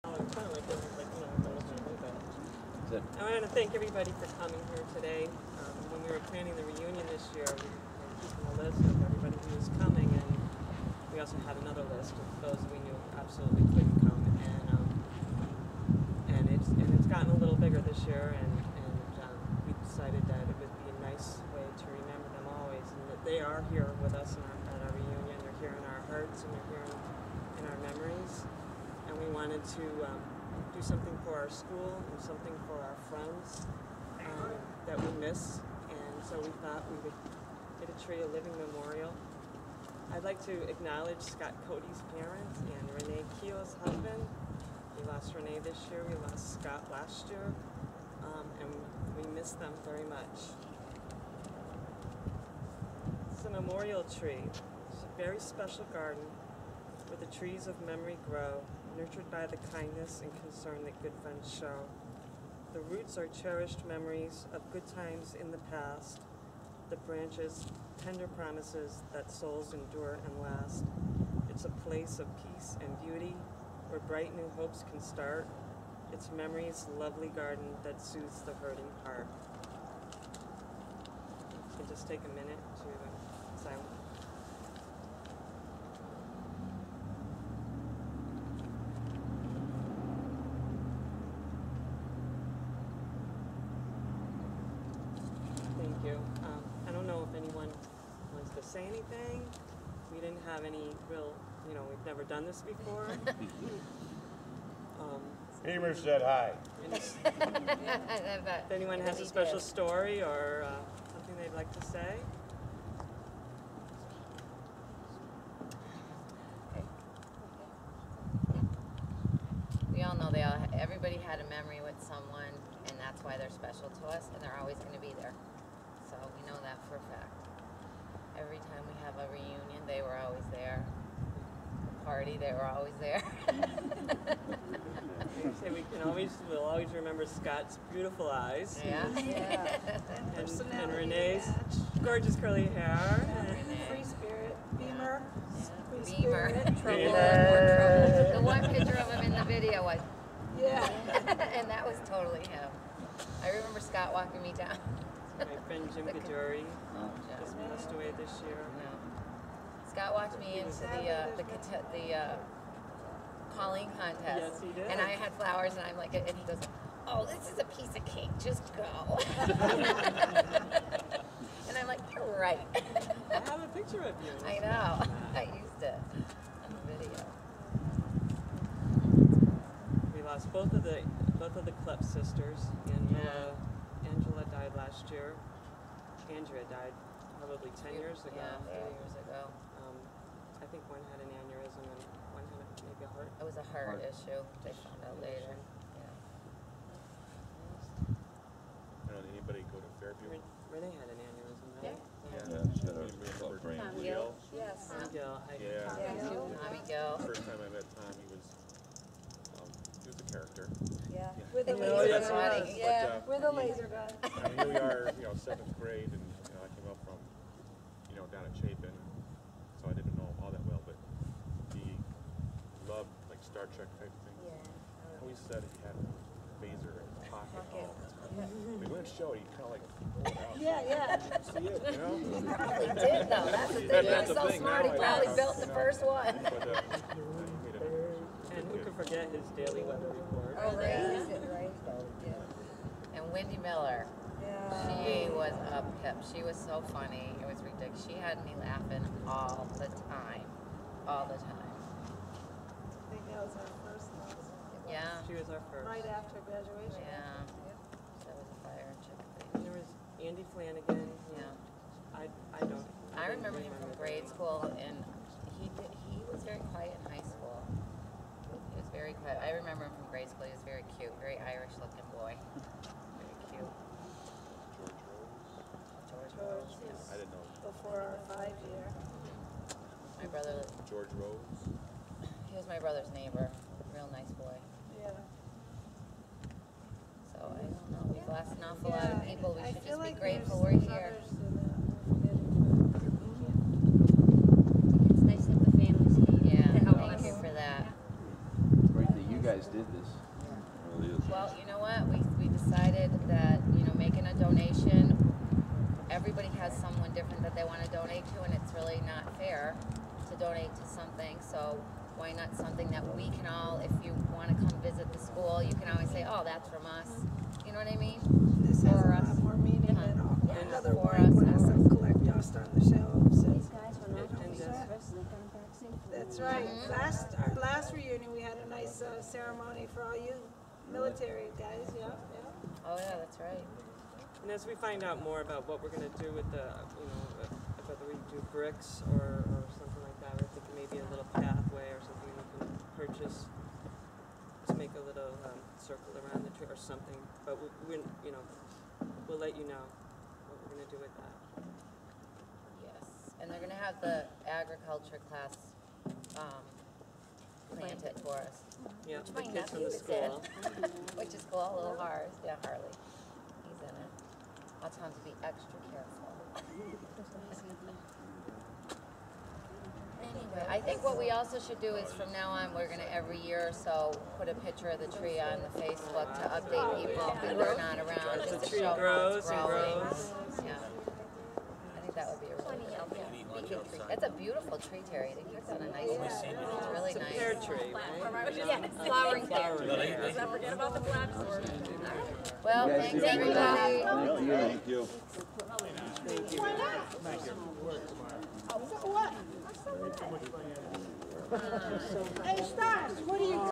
Chicopee Comp Class of 1982 30 Yr Reunion-Memorial Tree Ceremony
This is the short ceremony held on Sat, Oct 13, 2012 in front of the new high school to keep the memory alive of our classmates who have left us too soon.  This memorial tree will be planted on site next to the entrance of the football field.